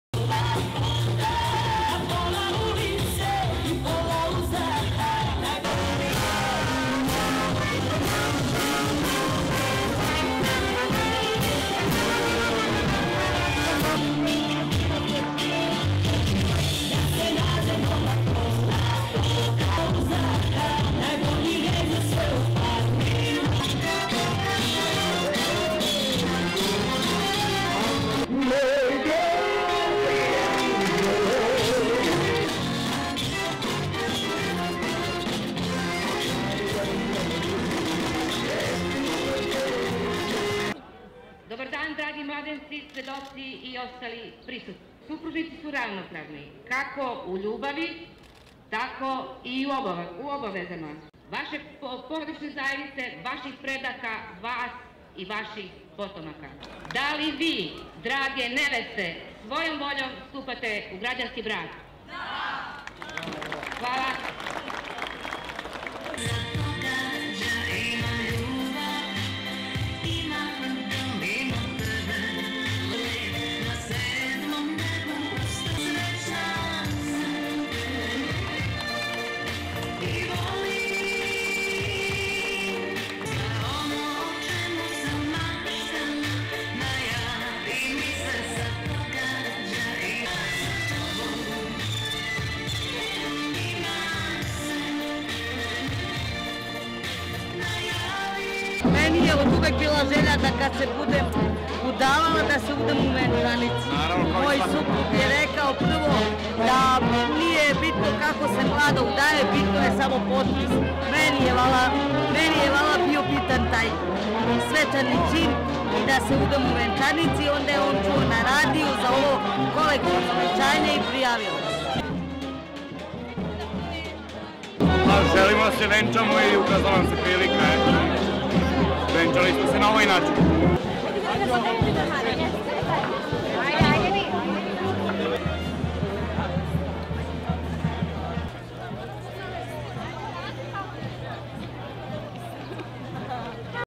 Dozens of couples tie knot at mass wedding in Belgrade
(22 May 2016) MASS WEDDING IN BELGRADE
About 50 couples tied the knot in a mass wedding in Serbia's capital Belgrade on Sunday.
As a band played, couples gathered in front of the city's town hall with brides dressed in glamorous white dresses.
It was the 18th time the event, organised by Belgrade town hall, has been held in the city.
After the ceremony the couples danced to music.